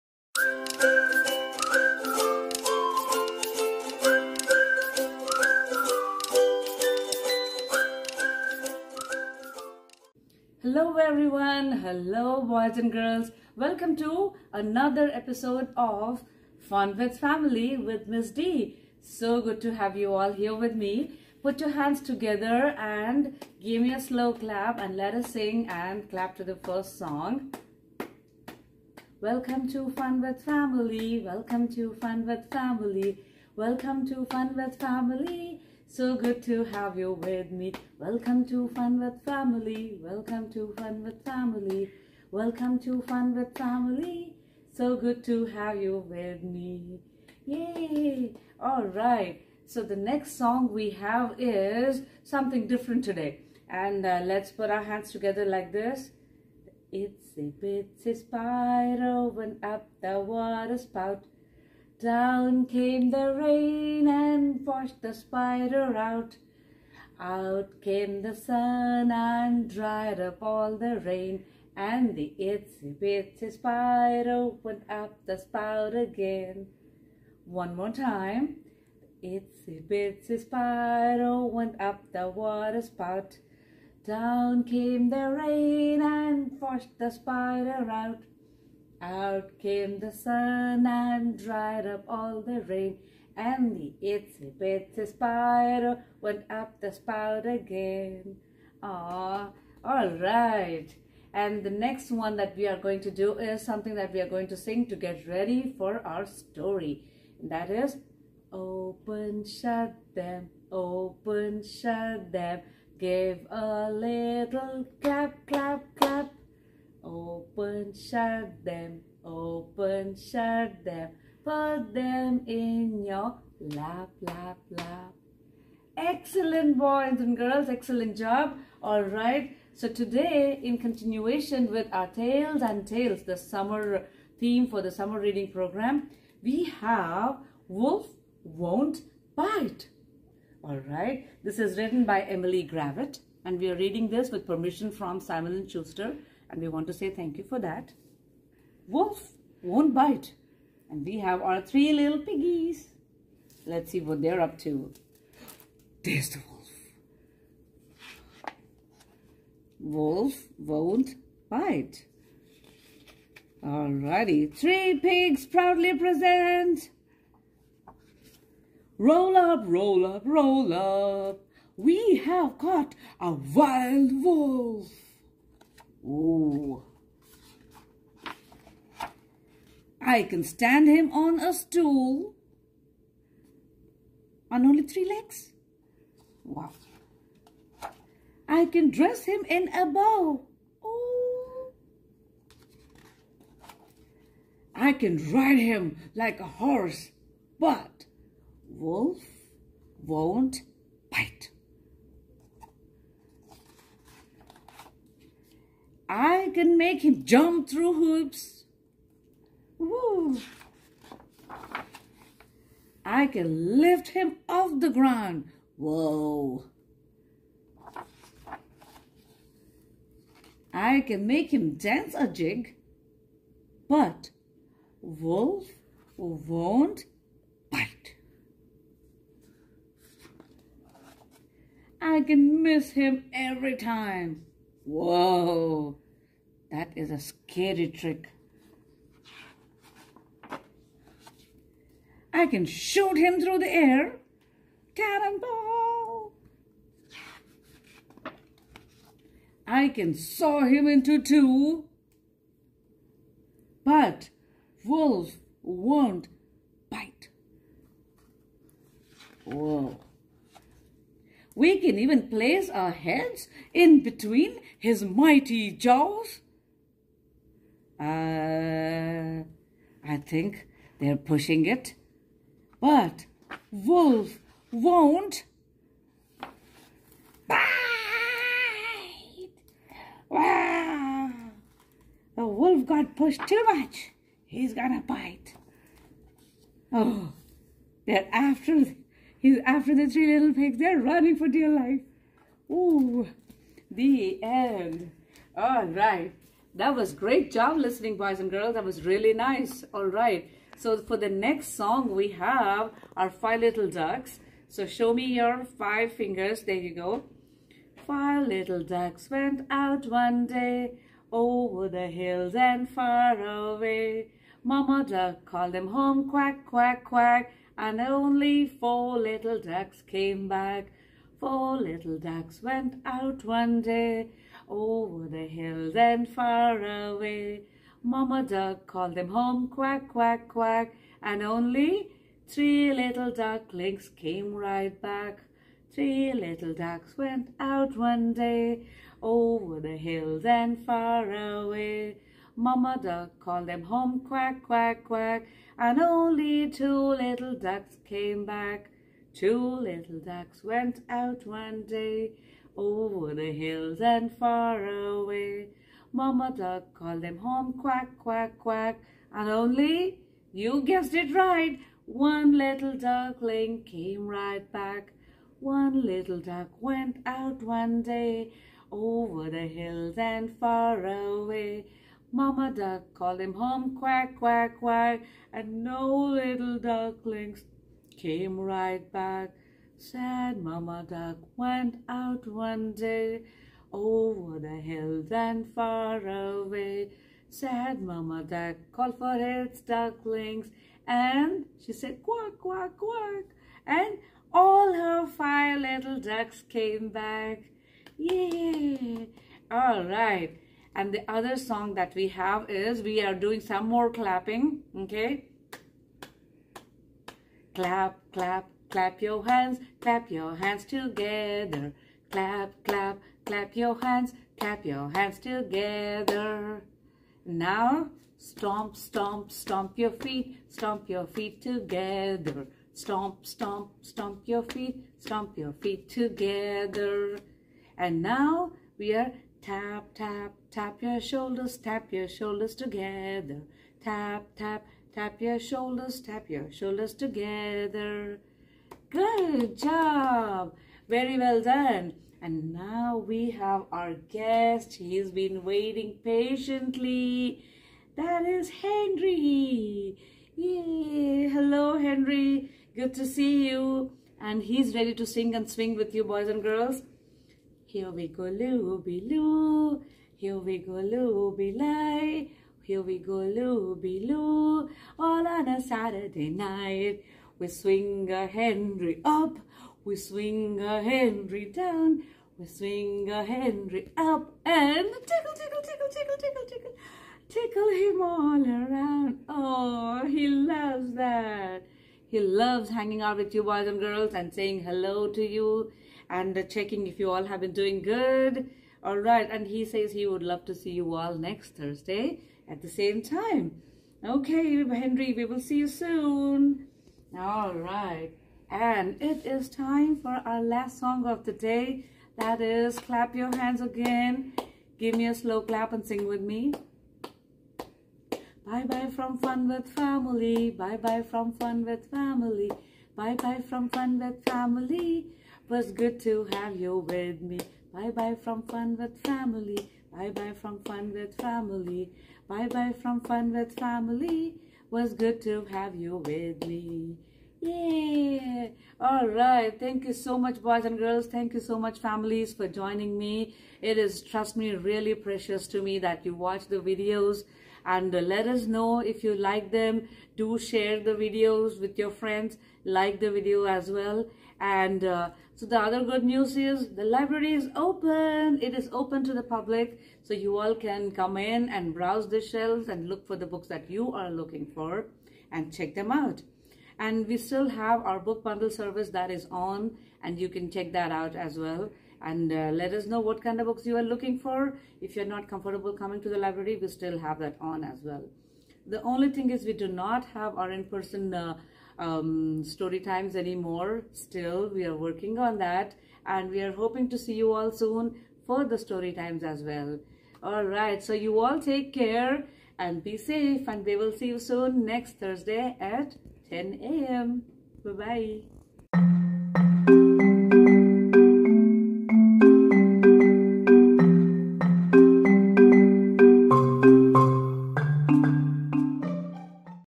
Fun with Family - Summer 2021 - Ep.6
Enjoy a Tale with Ms. D in another episode of Fun with Family.  Sing and clap along with a fun story and songs for all ages to enjoy.